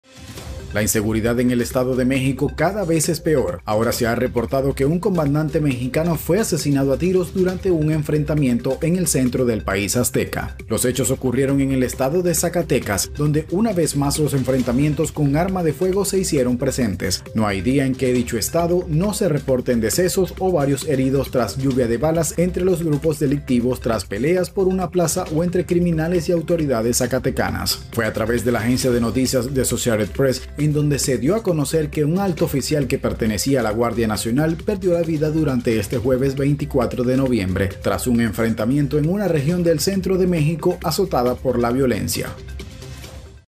Muere comandante mexicano en enfrentamiento contra el crimen organizado
Matan a comandante mexicano. La inseguridad en el estado de México cada vez es peor, la ola de violencia permanece en todos los estados de la República pero cabe mencionar que en ciertos lugares en especial es donde se ha desatado con más frecuencia, dejando las calles con muchas escenas aterradoras y llenas de sangre.